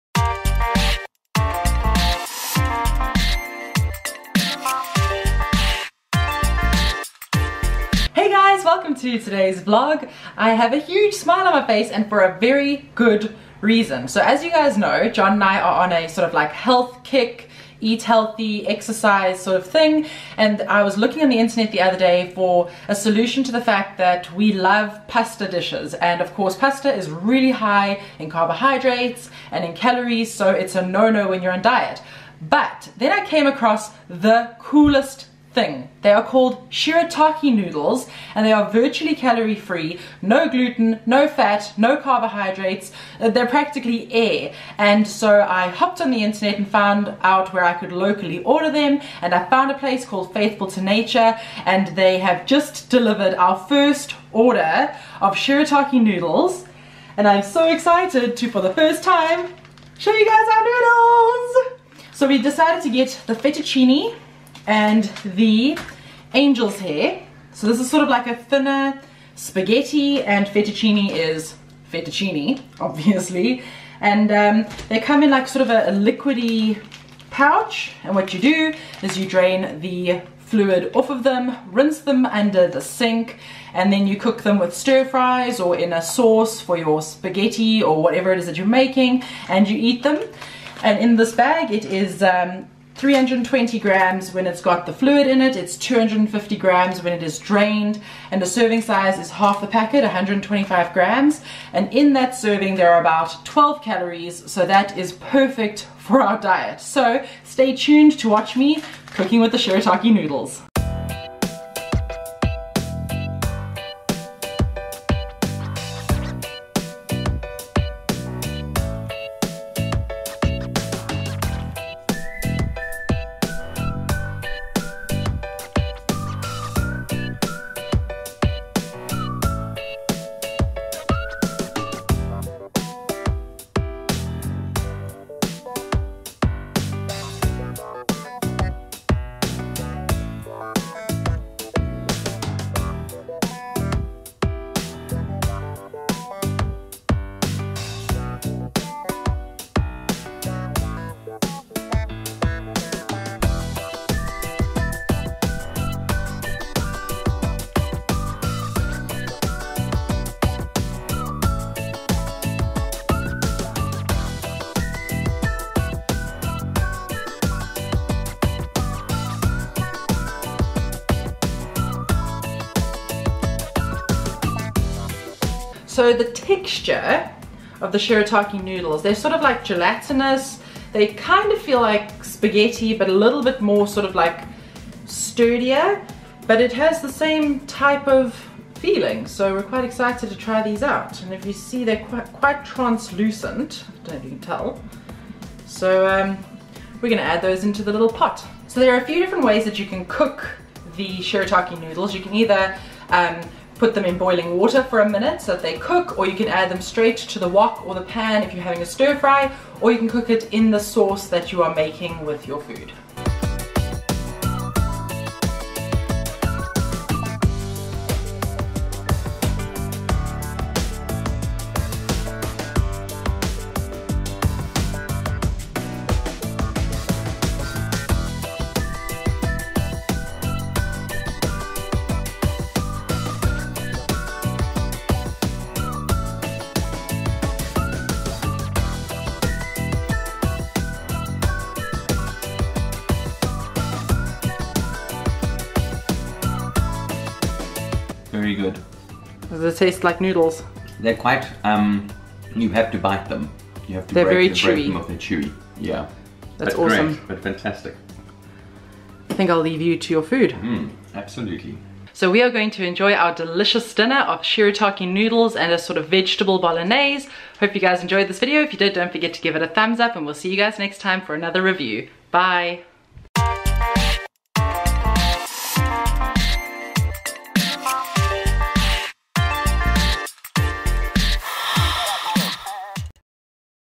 MAGIC SHIRATAKI NOODLES!
Top South African Vloggers, Thalia and John, do a product review of Superlite Shirataki Noodles!

OPEN ME!!!